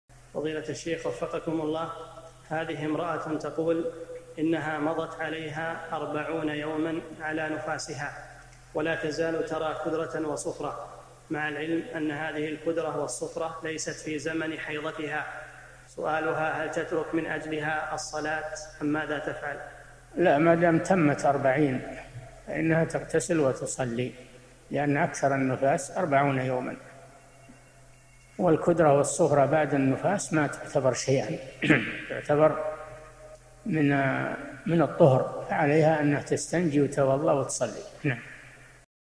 [188 /1350] امرأة مضى عليها أربعون يومًا على نفاسها، ولا تزال ترى كدرةً وصفرة..؟ الشيخ صالح الفوزان
[188 /1350] السؤال: هذه امرأة تقول: أنها مضت عليها أربعون يومًا على نفاسها، ولا تزال ترى كدرةً وصفرة، مع العلم أن هذه الكدرة والصفرة ليس في زمن حيضتها، سؤالها: هل تترك من أجلها الصلاة أم ماذا تفعل؟
 الجواب: لا، ما دام تمت أربعين؛ فإنها تغتسل وتصلي؛ لأن أكثر النفاس أربعون يومًا، والكدرة والصفرة بعد النفاس ما تعتبر شيئًا، تعتبر من الطهر، عليها أنها تستنجي وتتوضأ وتصلي.
للاشتراك في قناة الشيخ صالح الفوزان
أسئلة وأجوبة من شرح قاعدة جليلة ( 1350 ) مقطع

للاستفسار والإقتراحات والدعم
رئيس الفريق
لمشروع كبار العلماء